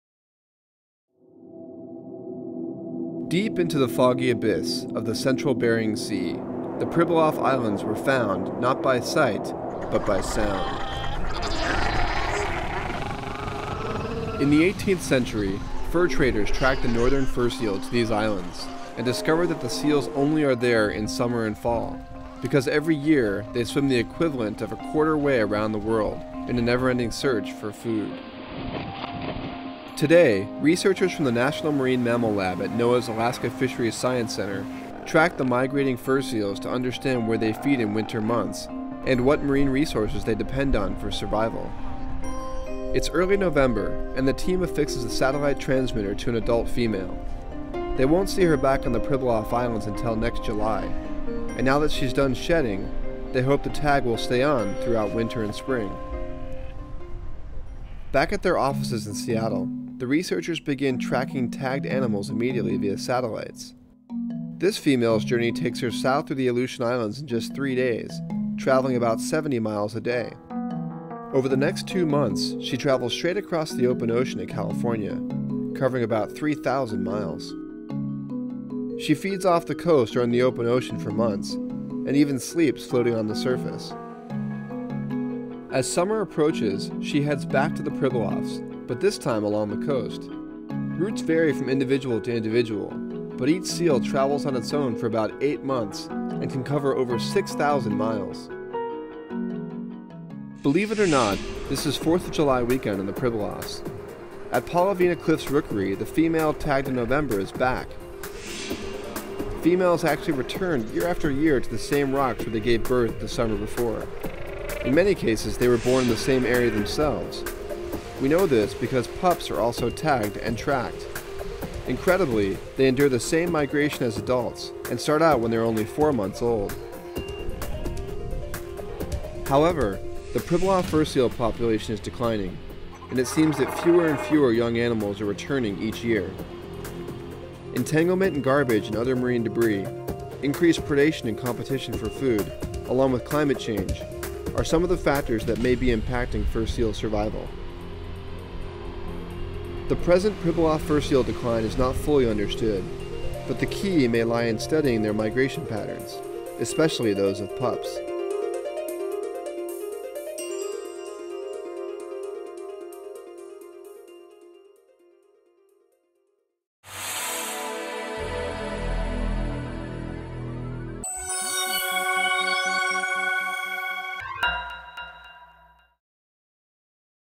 Northern Fur Seal Migrations
Every year, northern fur seals migrate 6,000 miles round-trip from their summer breeding grounds on the Pribilof Islands in the Bering Sea.  Learn about the scientists at NOAA who are tracking these seals on their incredible migrations, and where the seals go during the winter months!